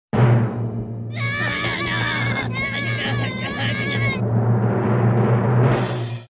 Stream Effects : DrumRoll
A drumroll in a new way... A stream favorite and great visual effect for viewers.